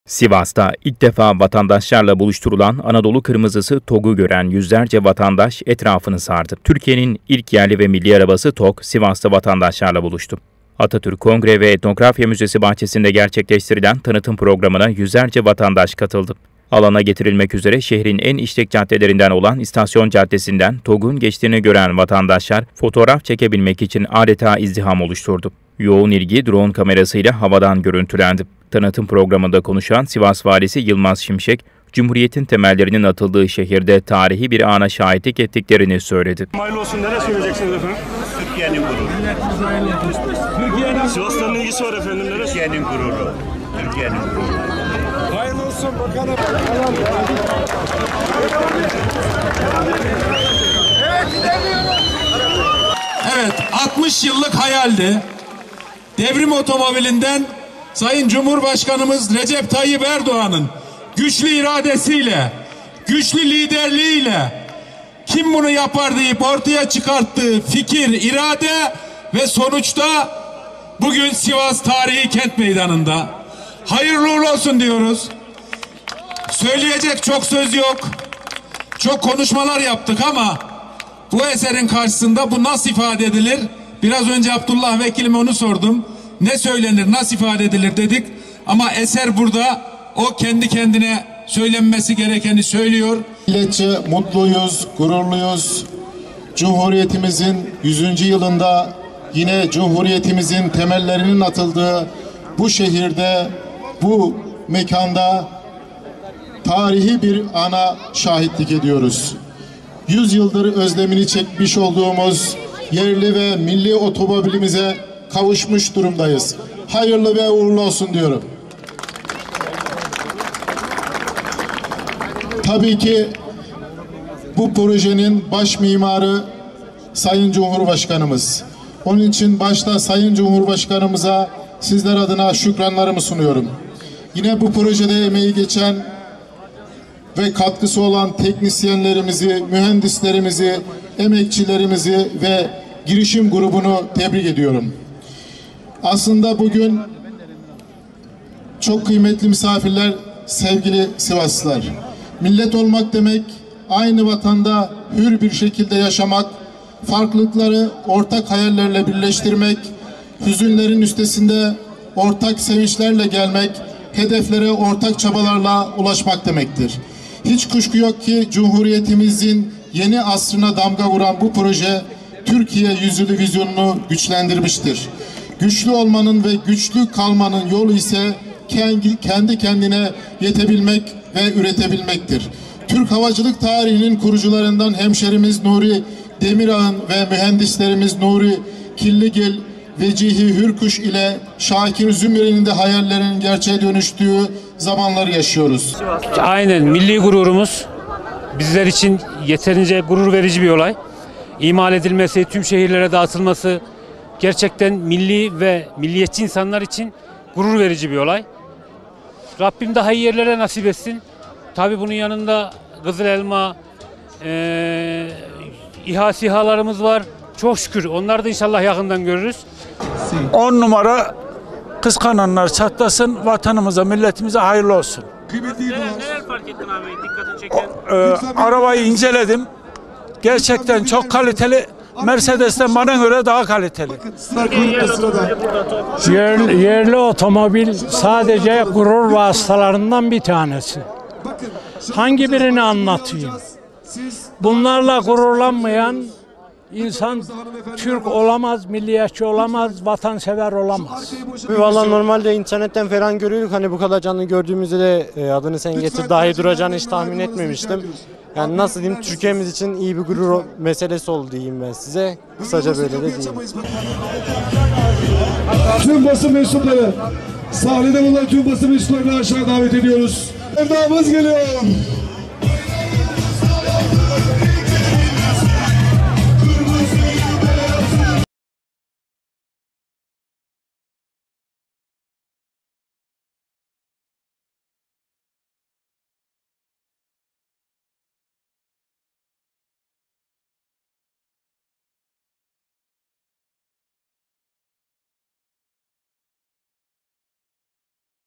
Togg İzdiham Oluşturdu O Anlar Havadan Görüntülendi
Sivas’ta ilk defa vatandaşlarla buluşturulan Anadolu kırmızısı Togg’u gören yüzlerce vatandaş etrafını sardı.

Türkiye’nin ilk yerli ve milli arabası Togg, Sivas’ta vatandaşlarla buluştu. Atatürk Kongre ve Etnografya Müzesi bahçesinde gerçekleştirilen tanıtım programına yüzlerce vatandaş katıldı. Alana getirilmek üzere şehrin en işlek caddelerinden olan İstasyon caddesinden Togg’un geçtiğini gören vatandaşlar fotoğraf çekilebilmek için adeta izdiham oluşturdu. Yoğun ilgi dron kamerası ile havadan görüntülendi. Tanıtım programında konuşan Sivas Valisi Yılmaz Şimşek, cumhuriyetin temellerinin atıldığı şehirde, tarihi bir ana şahitlik ettiklerini söyledi.

“Nuri Demirağ ve Nuri Hürkuş’un hayallerinin gerçeğe dönüştüğü zamanları yaşıyoruz”
Sivas Valisi Yılmaz Şimşek, güçlü olmanın ve güçlü kalmanın yolu kendi kendine yetebilmek ve üretebilmektir diyerek, “Milletçe mutluyuz, gururluyuz. Cumhuriyetimizin 100’üncü yılında yine cumhuriyetimizin temellerinin atıldığı bu şehirde, bu mekanda tarihi bir ana şahitlik ediyoruz. 100 yıldır özlemini çekmiş olduğumuz yerli ve milli otomobilimize kavuşmuş durumdayız. Hayırlı ve uğurlu olsun diyorum. Tabii ki bu projenin başpehlivanı Sayın Cumhurbaşkanımız onun için başta Sayın Cumhurbaşkanımıza sizler adına şükranlarımı sunuyorum. Yine bu projede emeği geçen ve katkısı olan teknisyenlerimizi, mühendislerimizi, emekçilerimizi ve girişim grubunu tebrik ediyoruz. Aslında bugün çok kıymetli, millet olmak demek aynı vatanda hür bir şekilde yaşamak, farklılıkları, ortak hayallerle birleştirmek, hüzünlerin üstünde ortak sevinçlerle gelmek, hedeflere ortakları ulaşmak demektir. Hiç kuşku yok ki cumhuriyet yeni yüzyılına damga vuran bu proje Türkiye vizyonunu güçlendirmiştir. Güçlü olmanın ve güçlü kalmanın yolu ise kendi kendine yetebilmek ve üretebilmektir. Türk havacılık tarihinin kurucularından hemşehrimiz Nuri Demirağ'ın ve mühendislerimiz Nuri Hürkuş’un hayallerinin gerçeğe dönüştüğü zamanları yaşıyoruz. İnşallah ülkemizin gururu bir Türk markası olarak yurt içinde ve dünyanın birçok ülkesinde yolları süsleyecektir” ifadelerini kullandı.

“100 yıl sonra en büyük Cumhuriyet eseri”
Sivas Belediye Başkanı Hilmi Bilgin, Cumhuriyetin kurulduğu Atatürk ve Kongre Binasının bahçesine 100 yılın en büyük eserinin getirildiğini belirterek, “Aslında bugün bizim konuşmamıza gerek yok. Konuşması gereken eser hepimizin gözünün önünde milletimize, dünyaya en güzel cevabı veriyor. Birileri konuşur, birileri söz verir ama eser Cumhuriyetin kurulduğu bu tarihi binanın yanında yüzyıl sonra en büyük Cumhuriyet eseri olarak milli ve yerli ilk otomobilimiz Sivas’a geldi. Şehrimize, ülkemize, milletimize hayırlı olsun. Evet, 60 yıllık hayaldi Devrim otomobilinden sonra Sayın Cumhurbaşkanımız Recep Tayyip Erdoğan'ın güçlü iradesiyle, güçlü liderliğiyle kim bunu yapar deyip ortaya çıkarttığı fikir, irade ve sonuçta bugün Sivas tarihi kent meydanında hayırlı uğurlu olsun diyoruz. Söyleyecek çok söz yok” şeklinde konuştu.
Öte yandan Togg'u gören vatandaşlar mutlu ve gururlu olduklarını ifade ettiler.